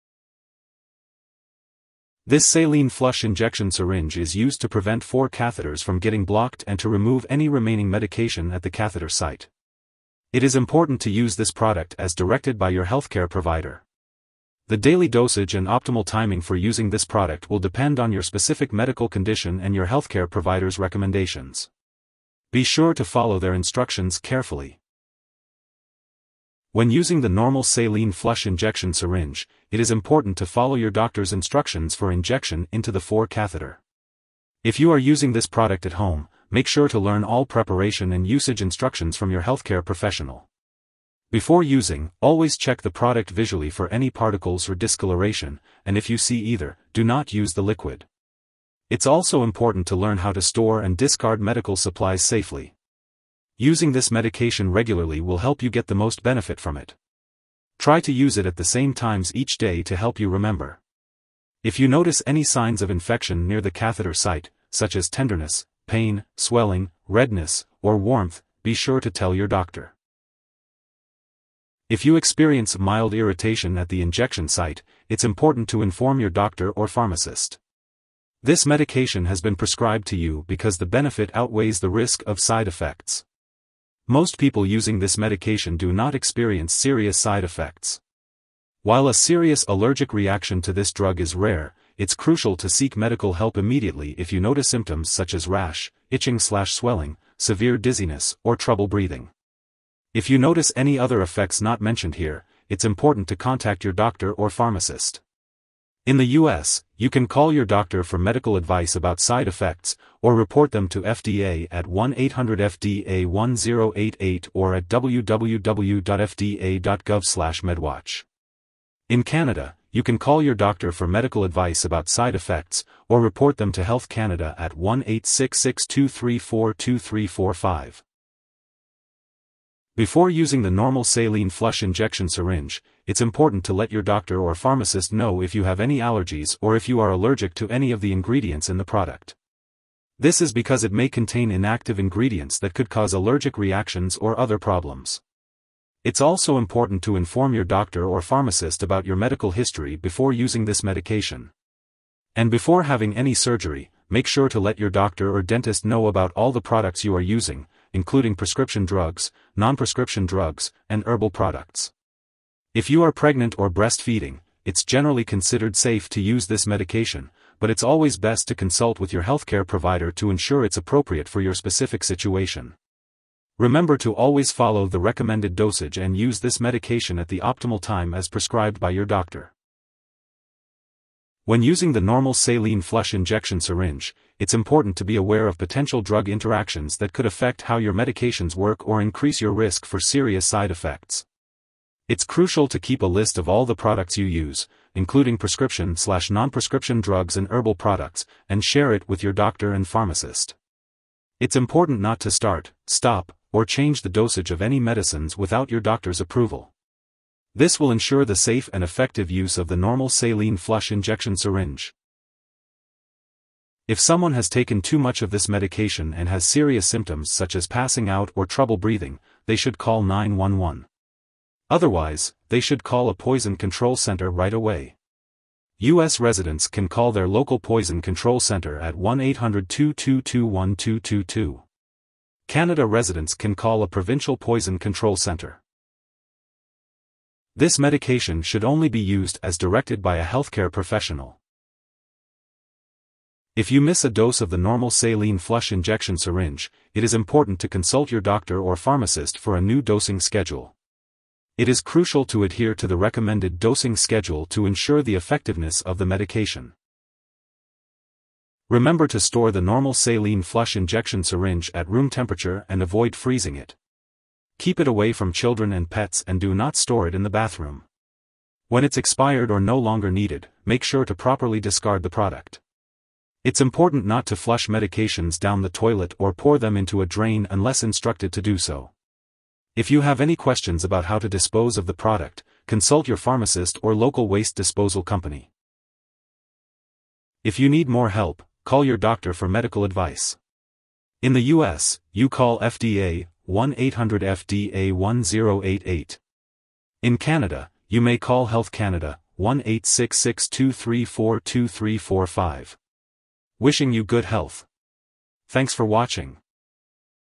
How to use Normal Saline Flush Injection Syringe - Explain Uses,Side Effects,Interactions
This saline flush injection syringe is used to prevent IV catheters from getting blocked and to remove any remaining medication at the catheter site. It is important to use this product as directed by your healthcare provider. The daily dosage and optimal timing for using this product will depend on your specific medical condition and your healthcare provider's recommendations. Be sure to follow their instructions carefully.
2e15